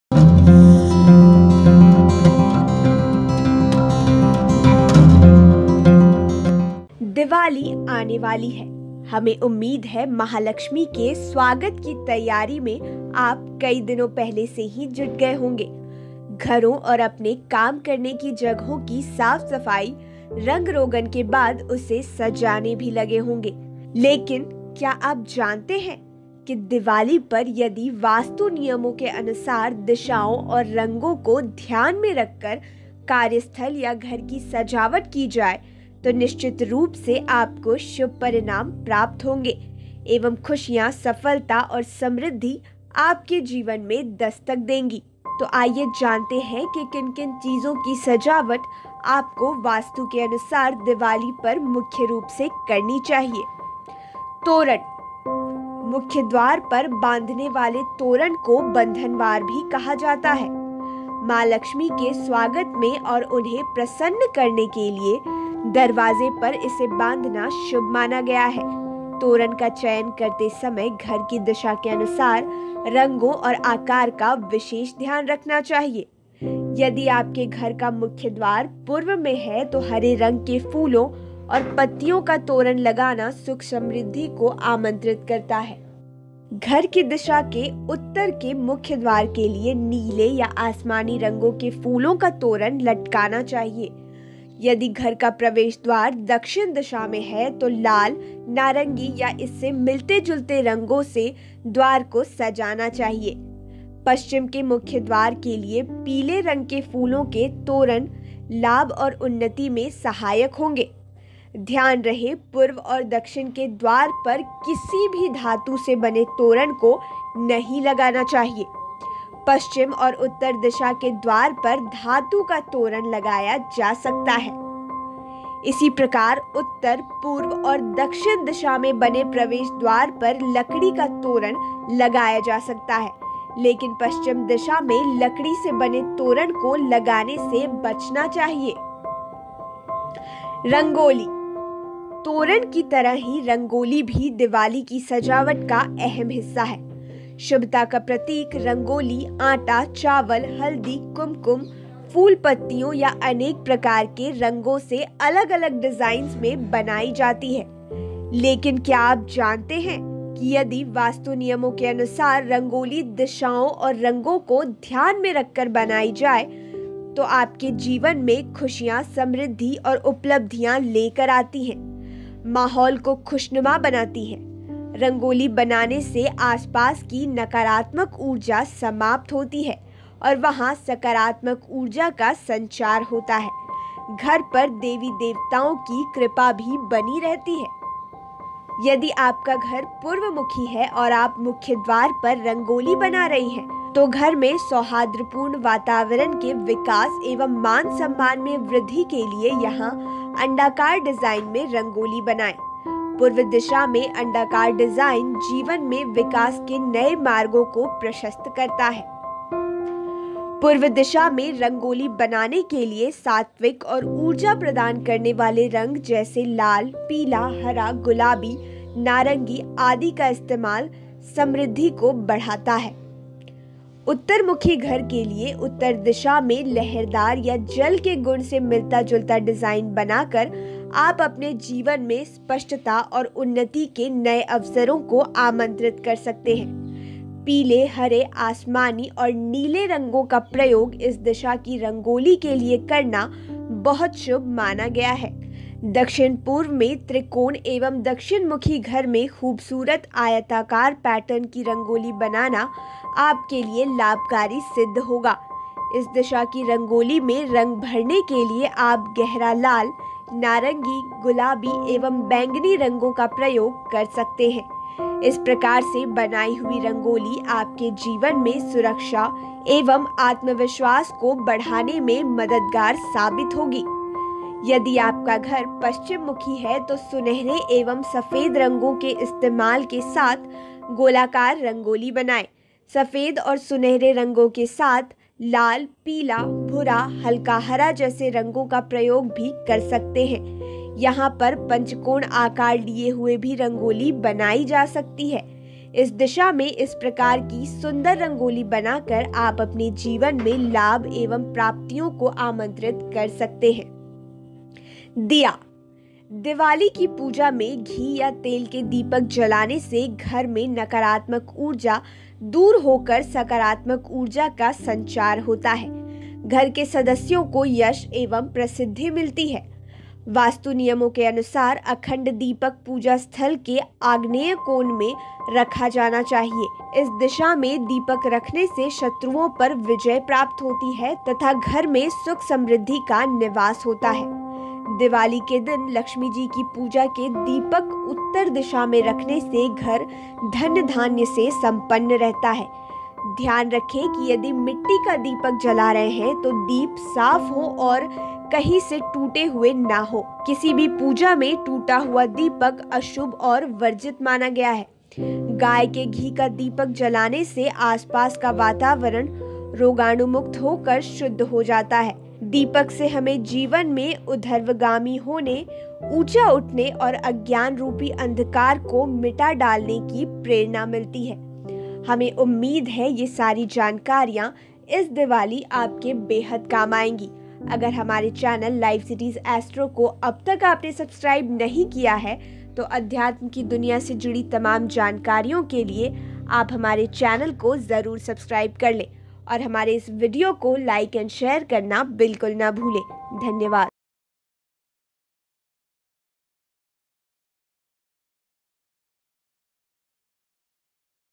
Diwali में इन Vastu Tips से दीजिए अपने घर को नया लुक, Maa Lakshmi भी हो जाएंगी खुश | Watch Video
Diwali में इन Vastu Tips से दीजिए अपने घर को नया लुक, Maa Lakshmi भी हो जाएंगी खुश | Watch Video | LiveCities